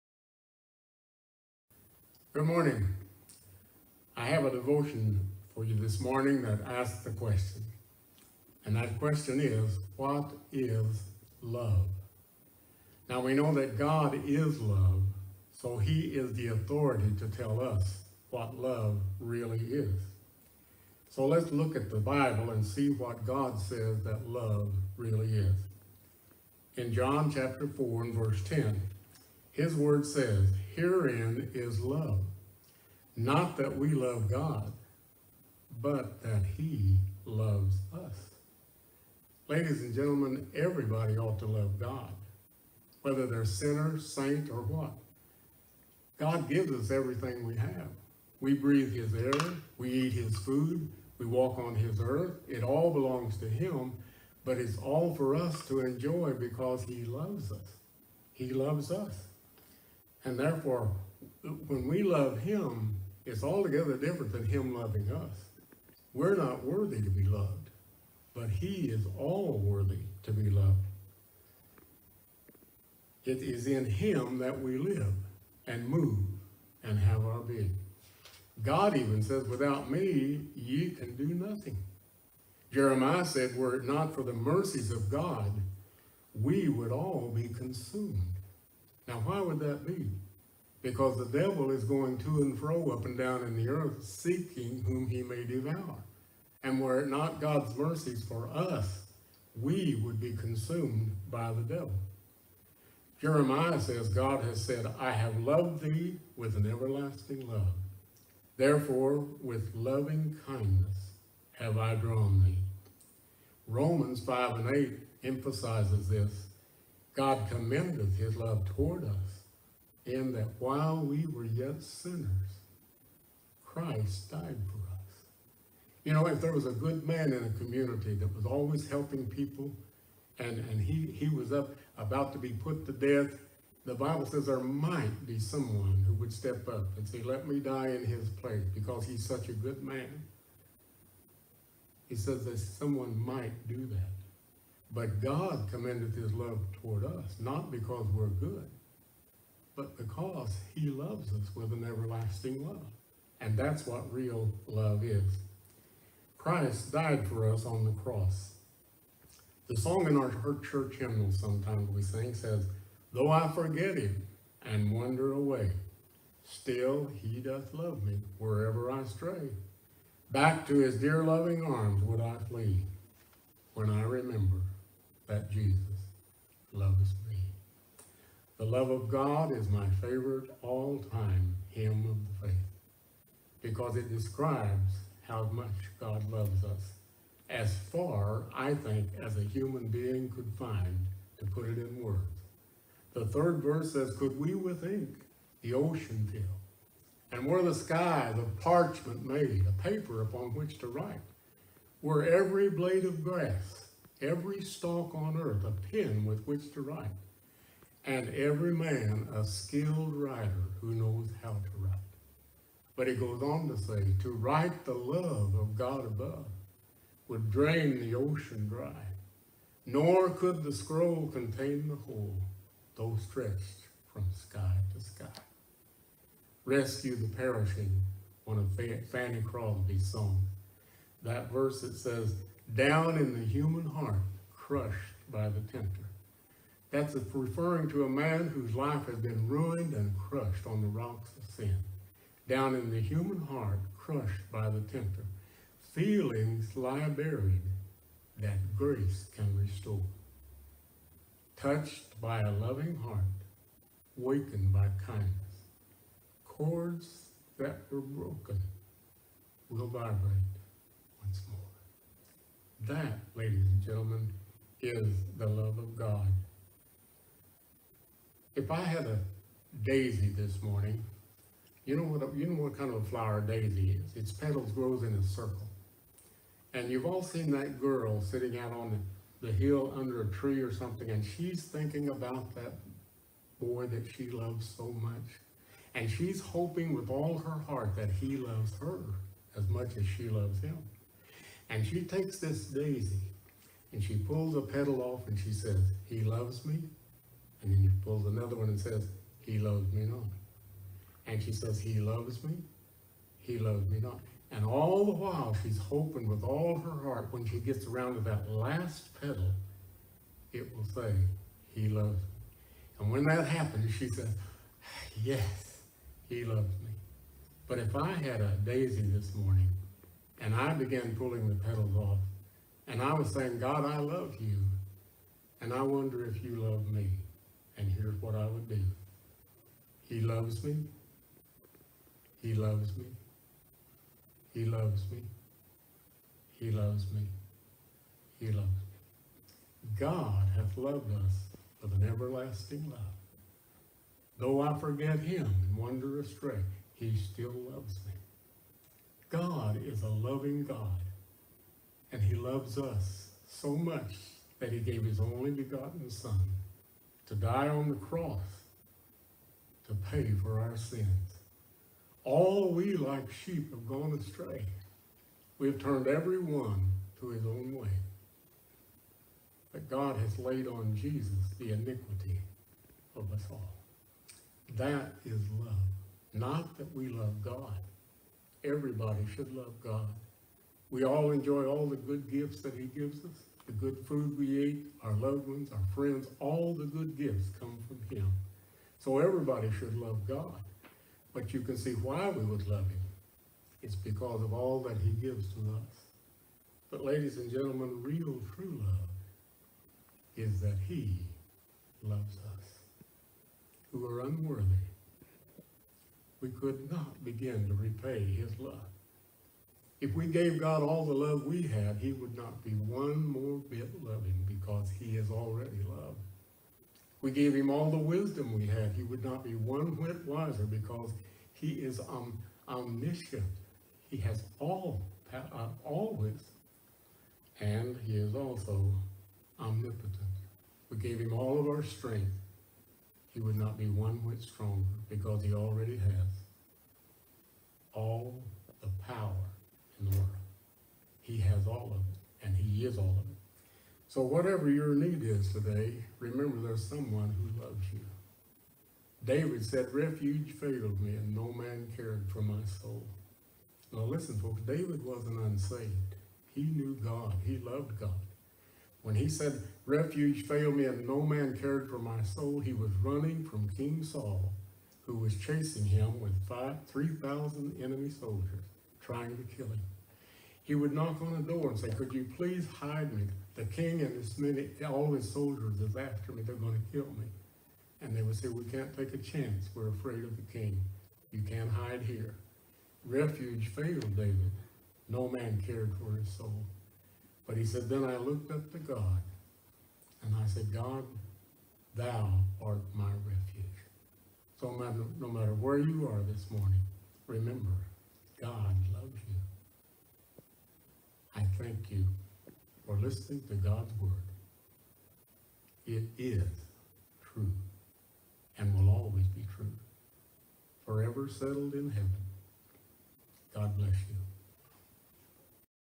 Joe Daniels 'The Love of God' Devotional - Heights Church of God - West Frankfort, Ill
Joe Daniels Devotion The Love of God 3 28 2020